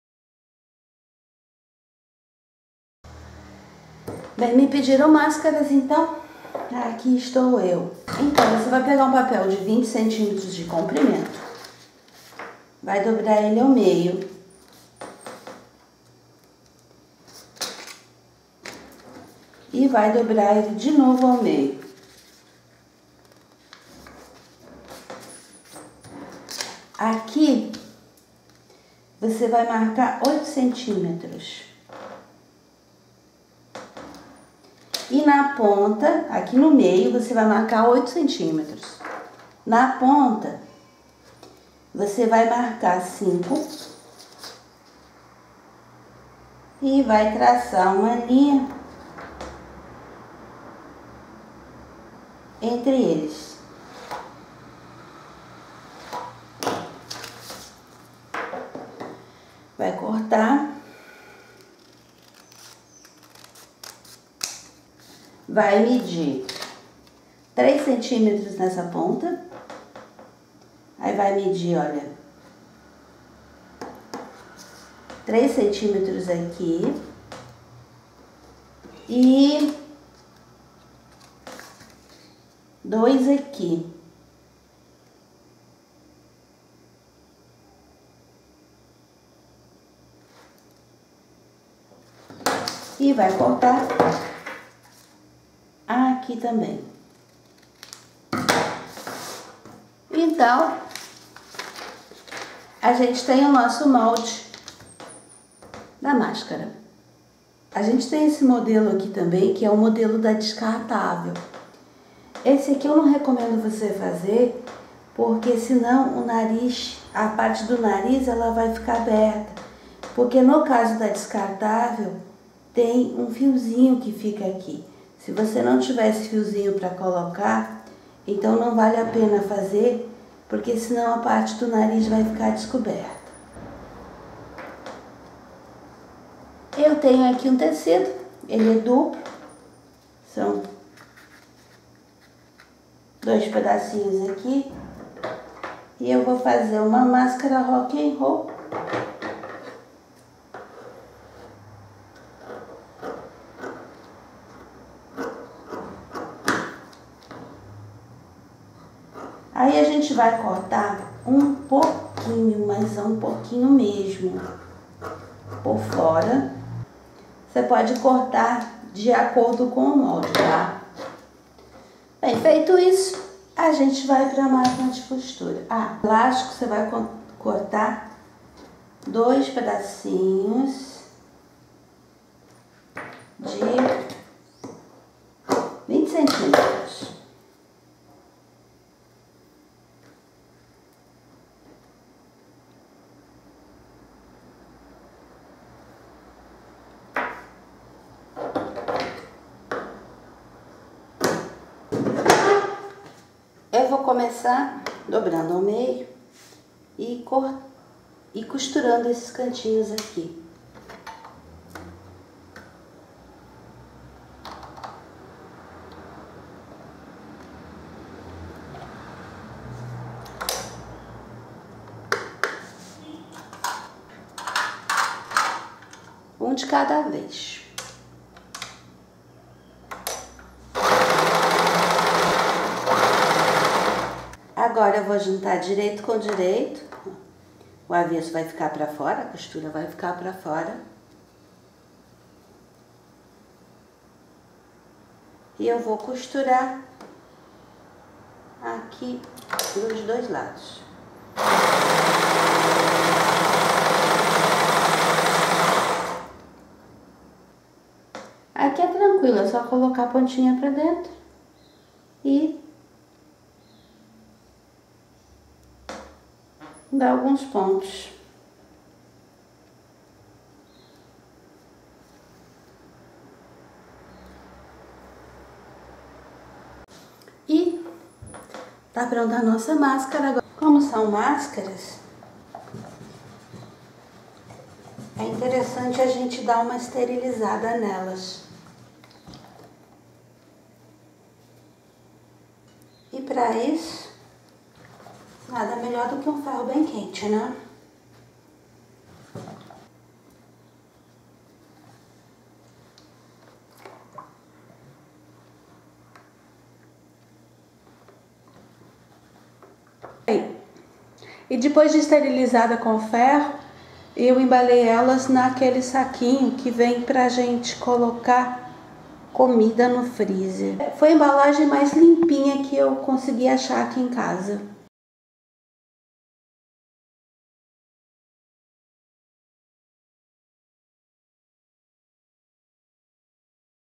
Como fazer mascara para enfrentar a pandemia
Uma maneira rápida e fácil de fazer uma mascara caseira para enfrentar esta pandemia que aí está.
a melhor saída é ficar em casa, mas se voce precisar ir a rua, melhor se proteger.